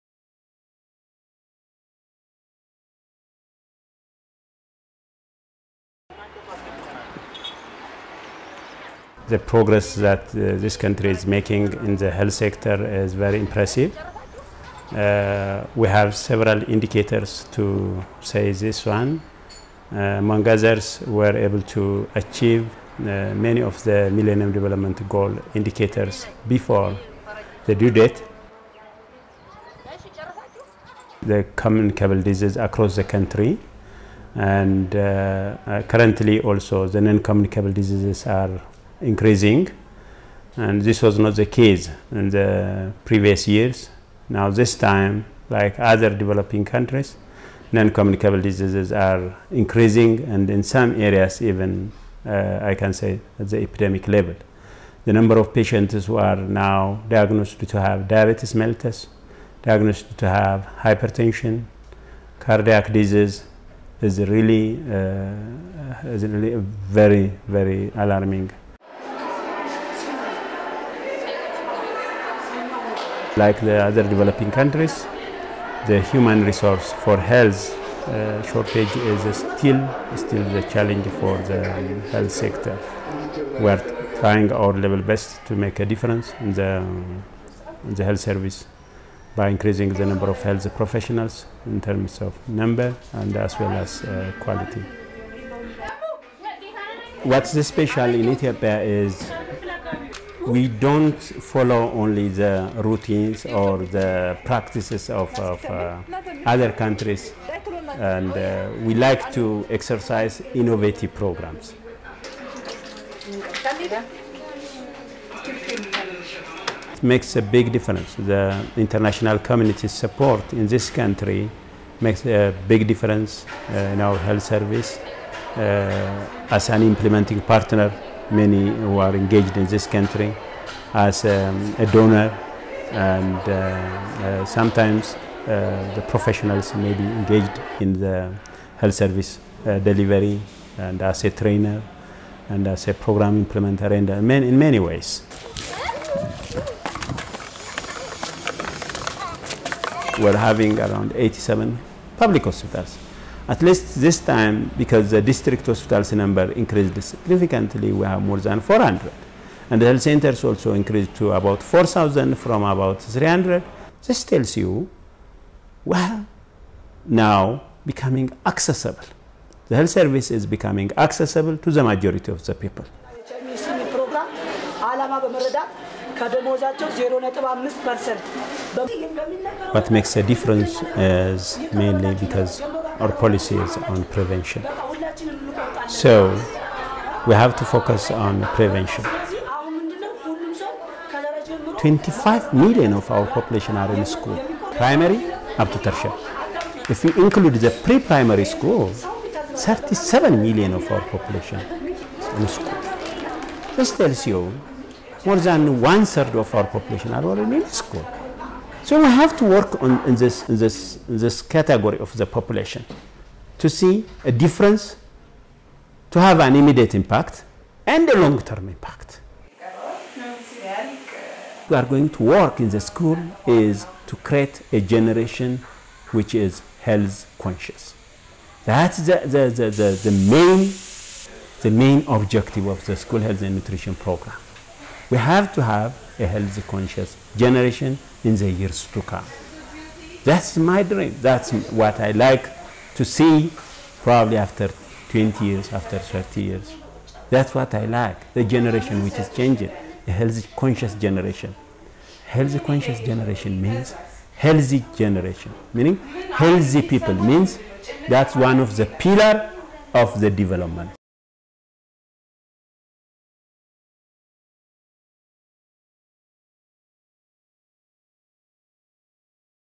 Etiopía. Entrevista a Yifru Berhan, Ministro de Salud etíope
El Ministro de Salud de Etiopía, Yifru Berhan, explica la situación de la salud pública en Etiopía y, en concreto, el programa de extensión sanitaria que permite el acceso a la sanidad básica a la población más vulnerable.

En Etiopía, más del 80% de la población vive en áreas rurales. Por ello, uno de los objetivos primordiales del plan de acción del Programa  de Extensión Sanitaria (en inglés, Health Extension Program) es la mejora del acceso en términos de igualdad a una cobertura sanitaria universal para llevar a cabo intervenciones básicas en pueblos y aldeas.